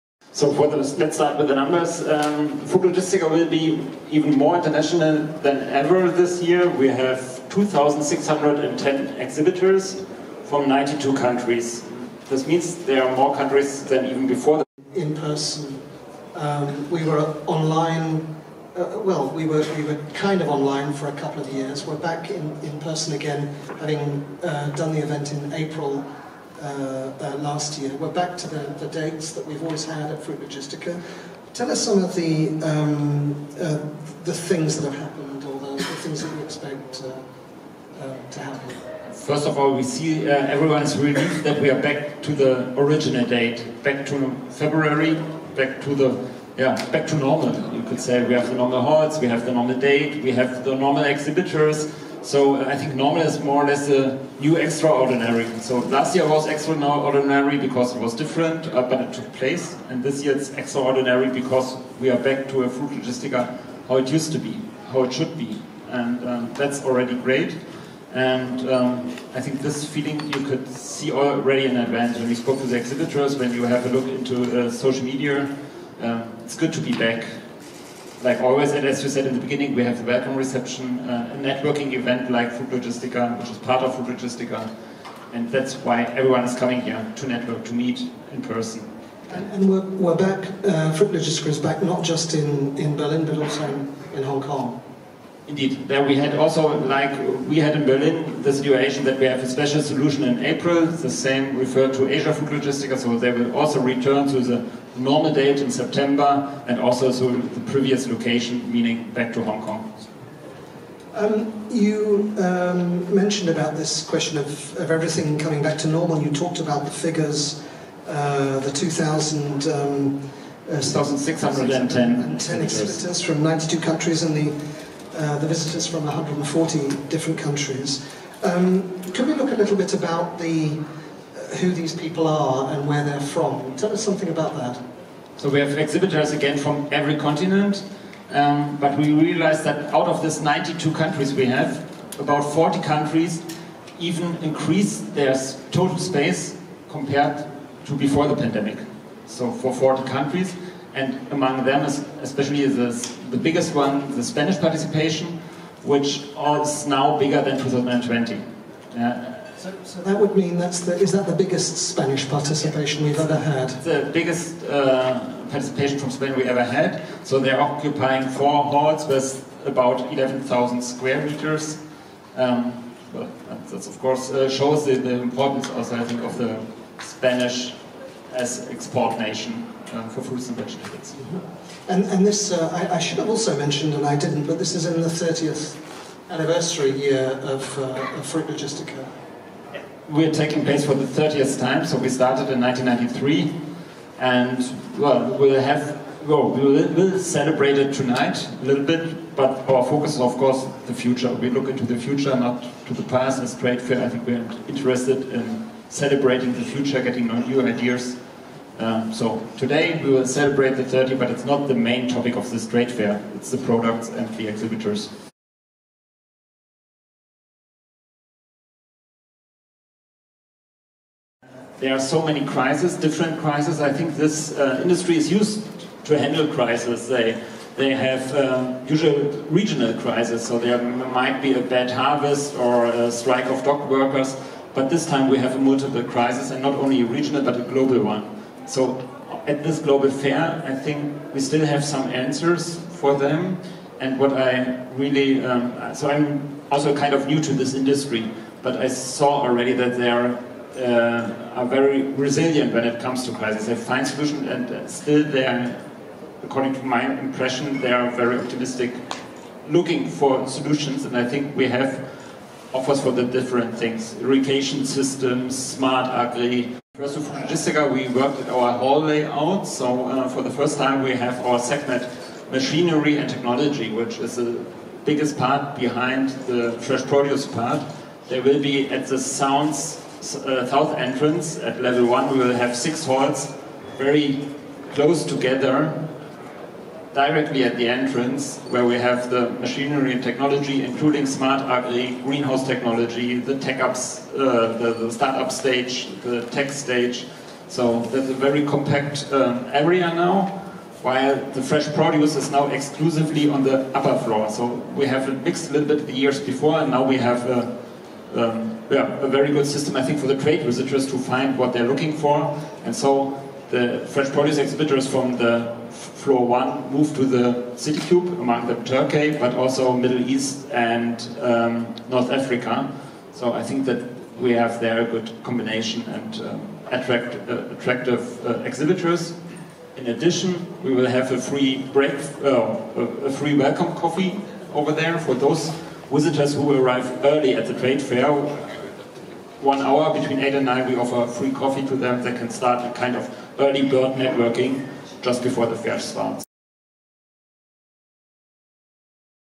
Fruit Logistica 2023 Berlin Information Highlights
Die FRUIT LOGISTICA ist die führende Fachmesse für den globalen Fruchthandel und bildet die gesamte Wertschöpfungskette der Obst- und Gemüsebranche.

FRUIT LOGISTICA findet vom 8. bis 10. Februar 2023 in Berlin statt.